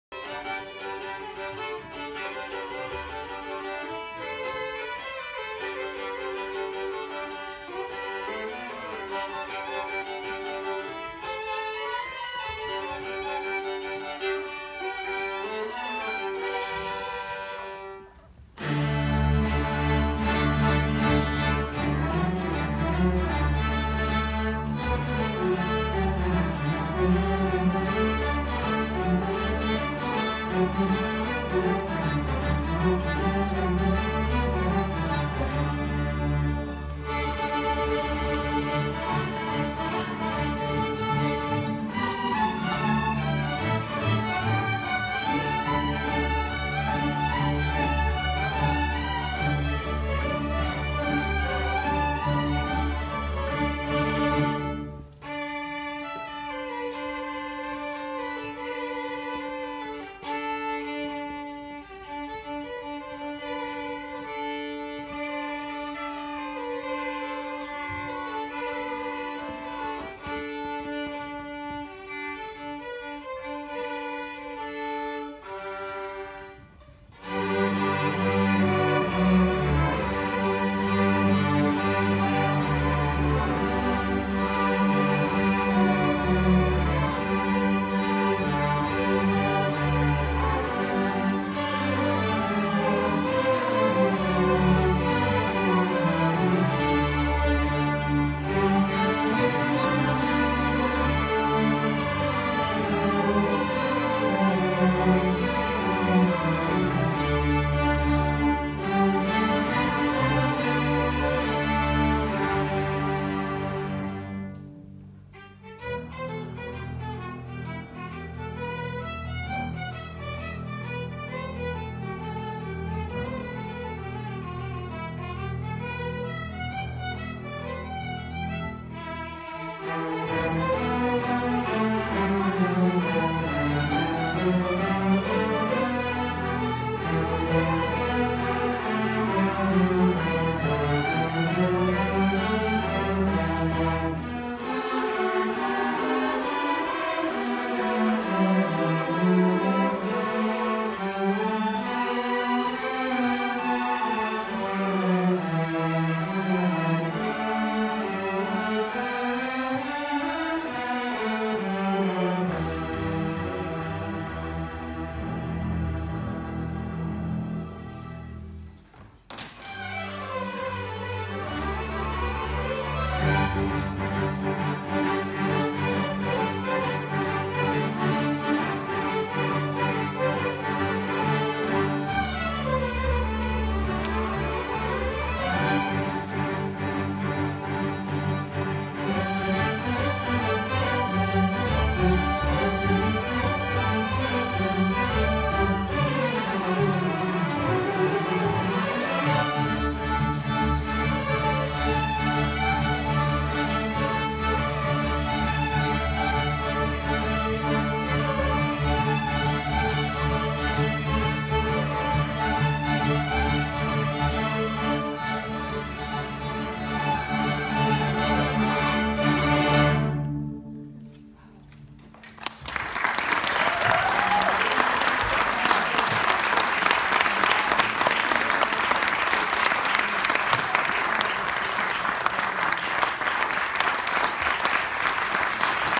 2018-12-17 - Katie Scott - viola - Christmas medley - Pelham Memorial High School Orchestra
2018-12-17 - Catherine Scott - viola - Christmas - Pelham Memorial High School Orchestra